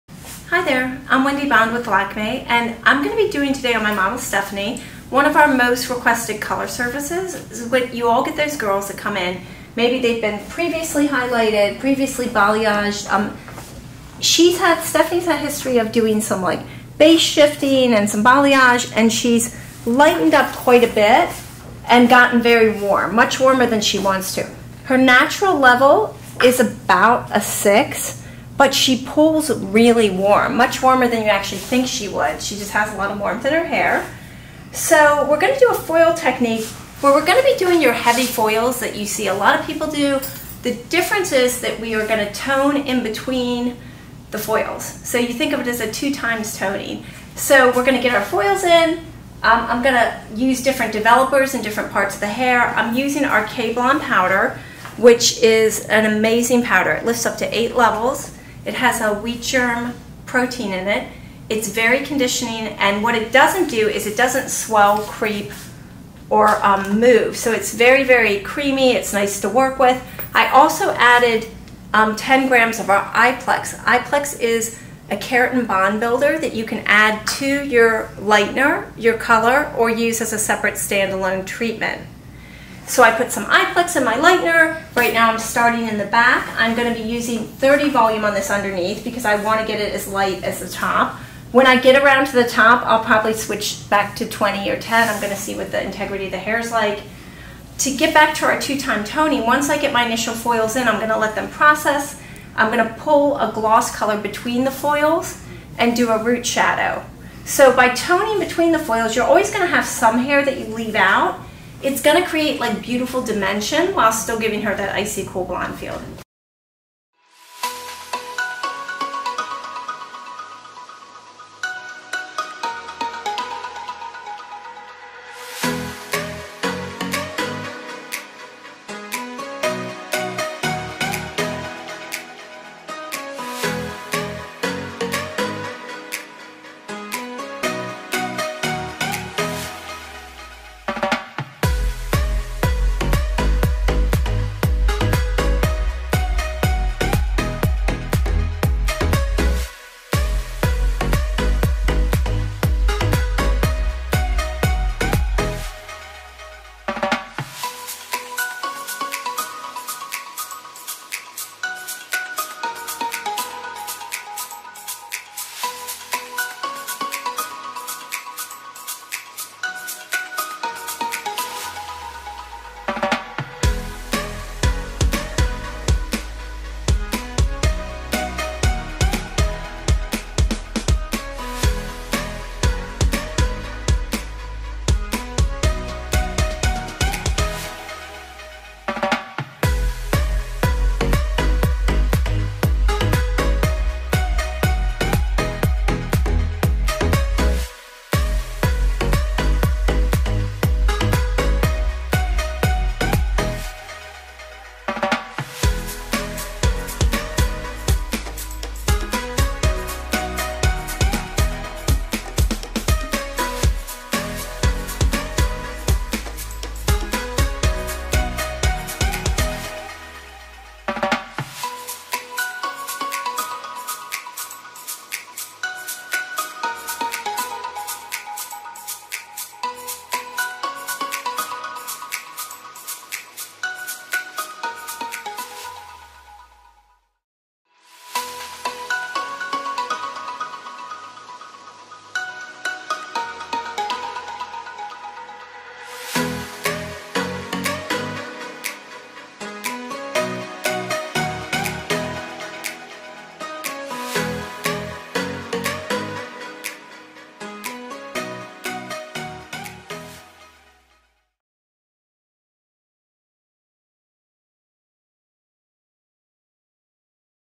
Toning Between The Foils For The Perfect Blonde
Watch how our National Director of Education Wendy Bond transforms her model into the perfect cool blonde by toning between the foils using our Lakme summer toners collection!